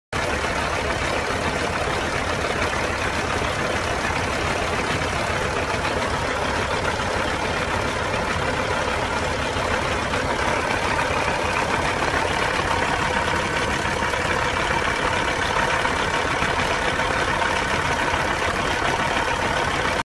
2004 FREIGHTLINER CENTURY 120 For Sale
This is a very unique truck with a big cat engine C 15
New motor was put in in 2020
All rebuilt
Excellent powerful truck on owned by  mechanic for the last 15 years. Only reason selling it due to issue with the family